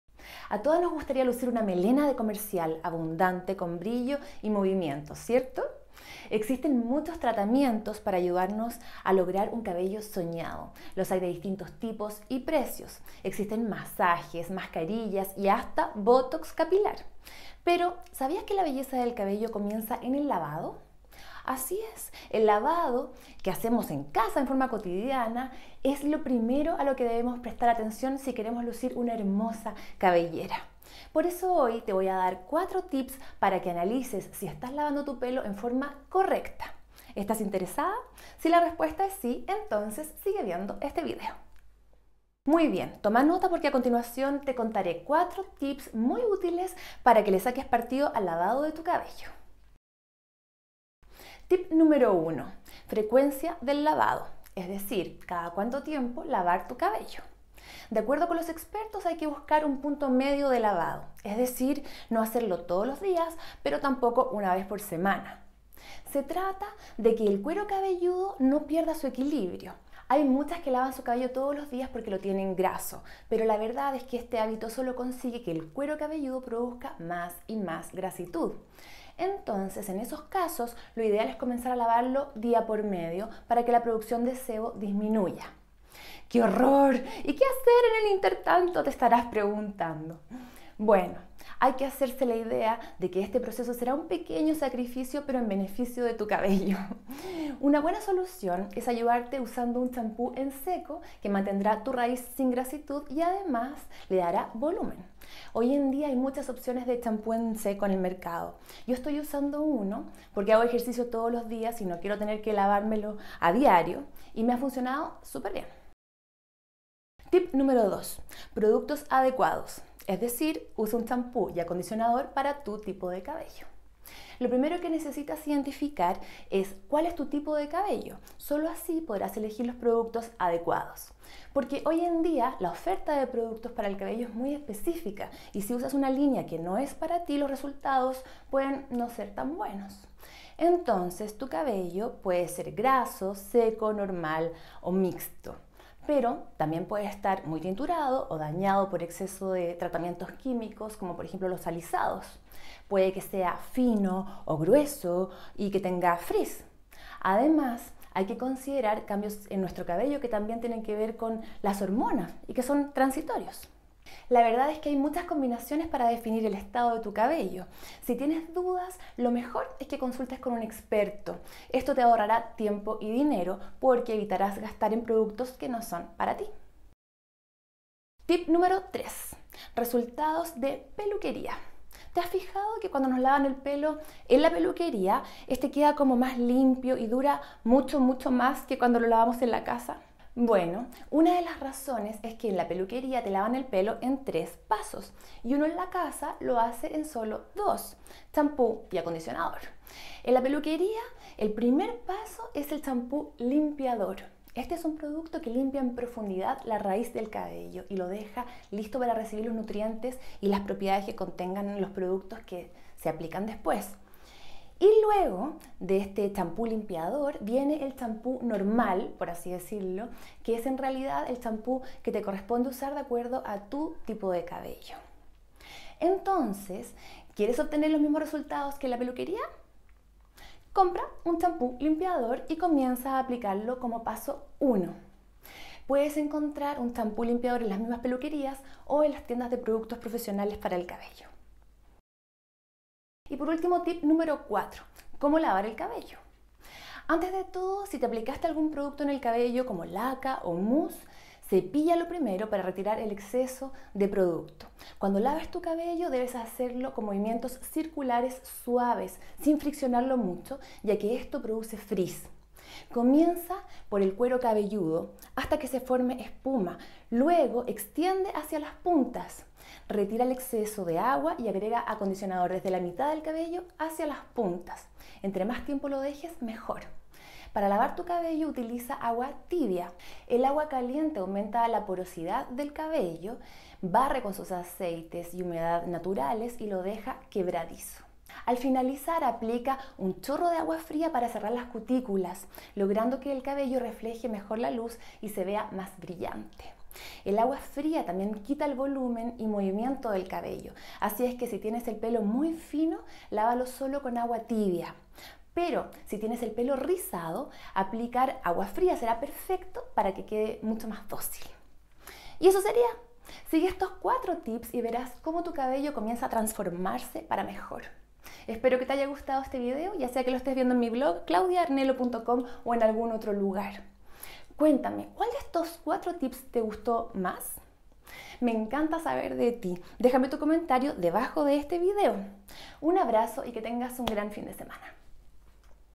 4 Tips para lavar tu cabello correctamente
¿Sabías que la belleza del cabello comienza en el lavado que hacemos en casa?
En el video de esta semana te daré 4 tips súper útiles para que te asegures de lavar tu cabello correctamente y le saques el máximo partido:
    - Frecuencia del lavado: cada cuánto tiempo lavar tu cabello
    - Productos adecuados: elige los productos indicados para ti.
    - Resultados de peluquería: cómo lograr resultados profesionales en casa.
    - Cómo lavar el cabello para que esté sano y hermoso.